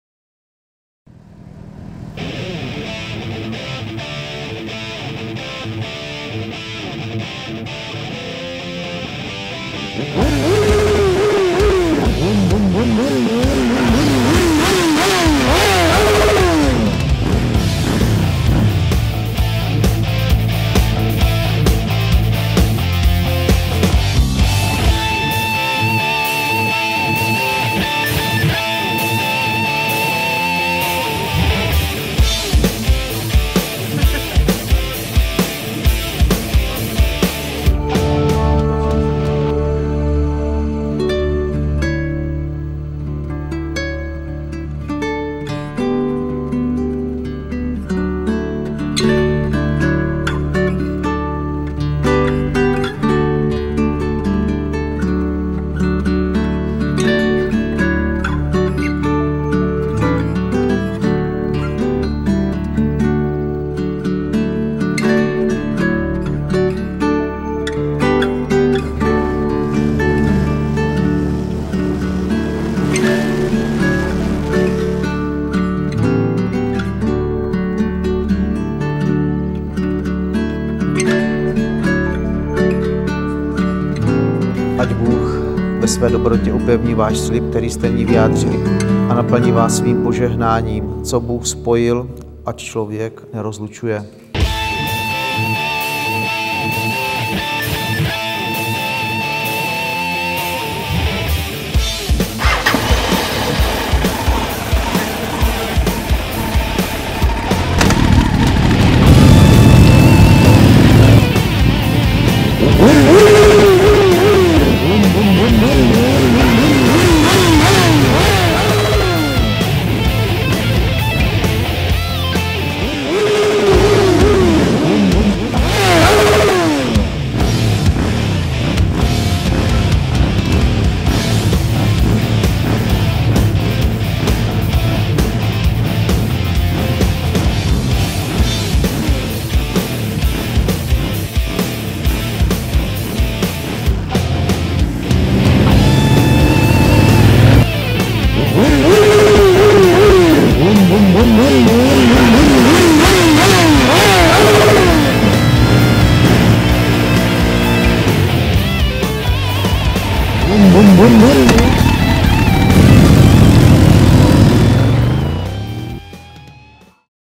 Katka & Martin - Svatební den
Red Love Wedding Trailer - motorbike wedding,
svatební den na motorkách,
libor svoboda film - liborsvoboda.cz